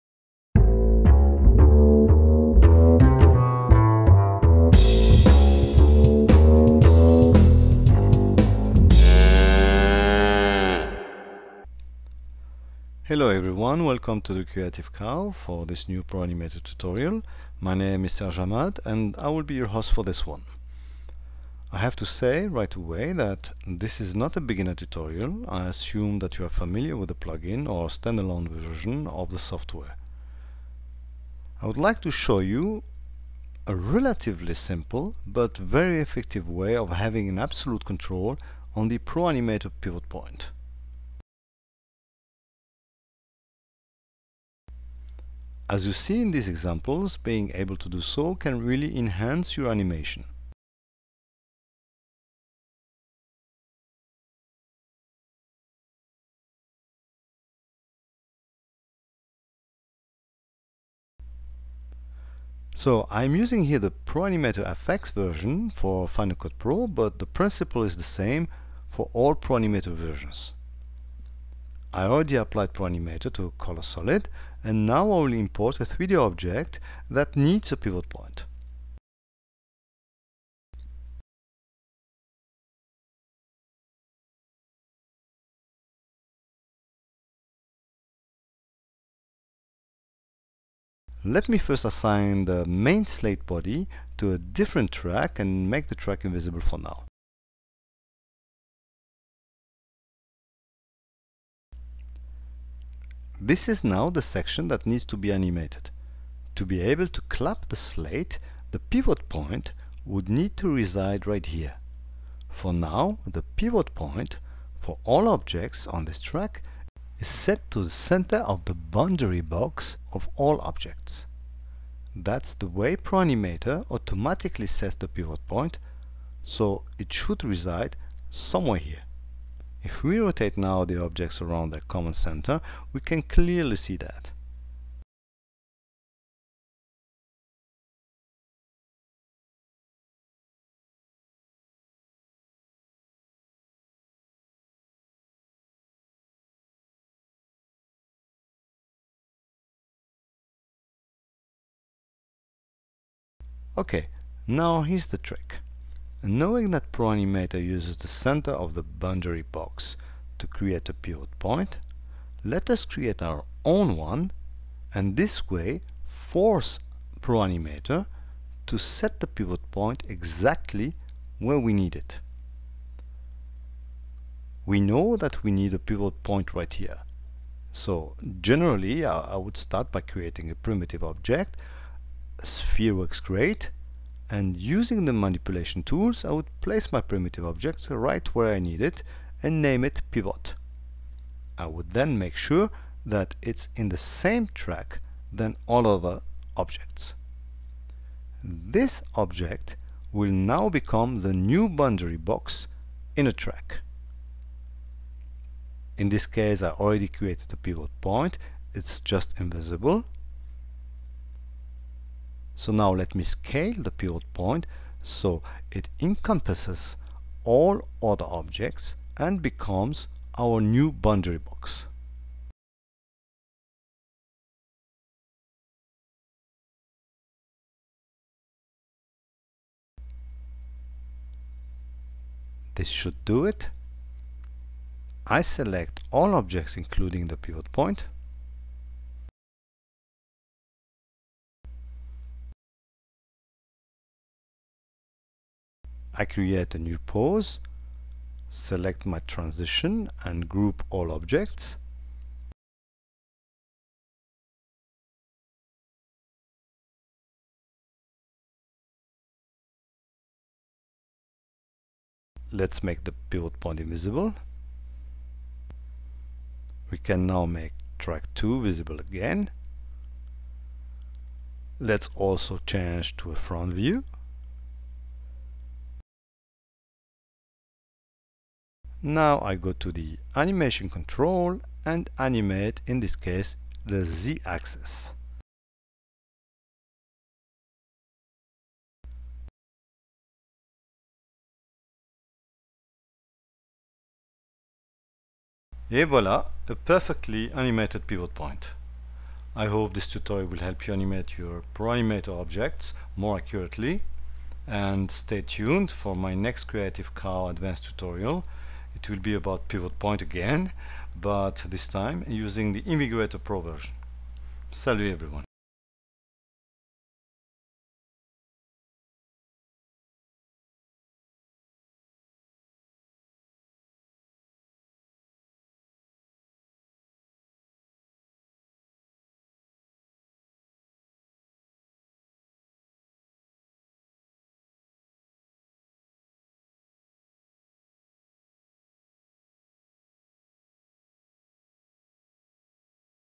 Controlling Zaxwerks ProAnimator Pivot-Point
Controlling ProAnimator's pivot point has always been a daunting task! In this video tutorial, Creative COW leader, Serge Hamad will share with you a simple but very effective trick to gain ABSOLUTE control over the Pivot-Point and consequently greatly enhance your ProAnimator animations.